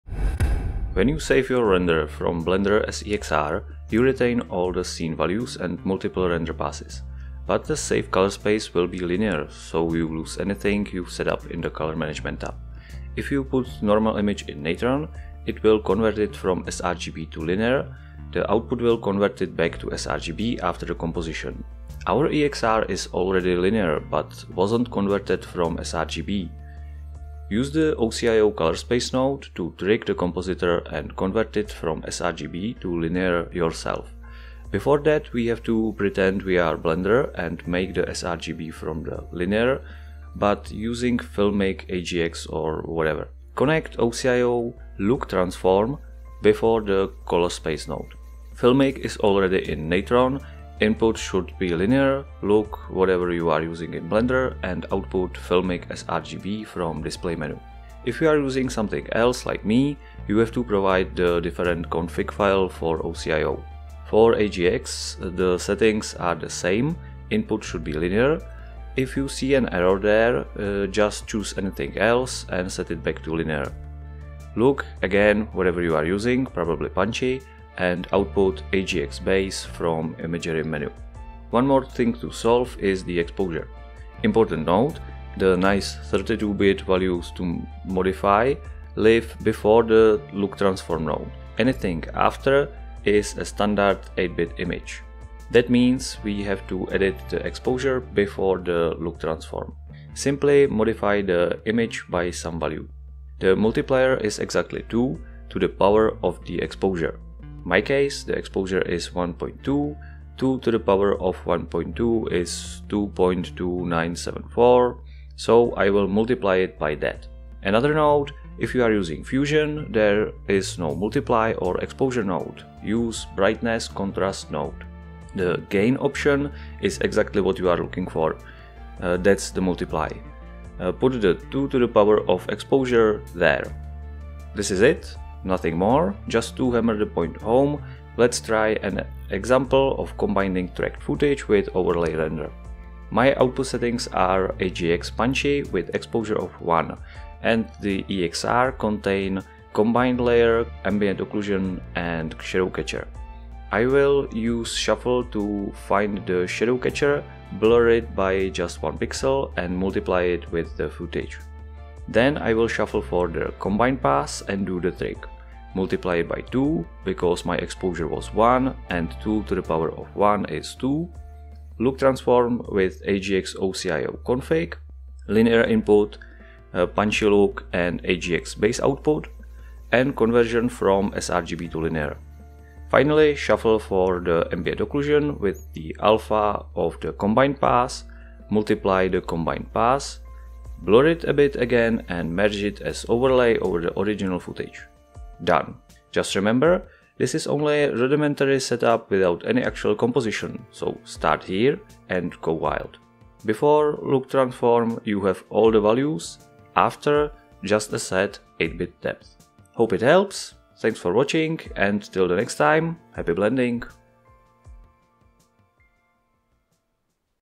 Filmic or AgX from Blender EXR to Natron or Fusion
This is just a reminder so I won't forget the basic setup.
It shows how to recreate Blender Color Management in other composition software and use it, for example, to combine tracked footage with rendered CG.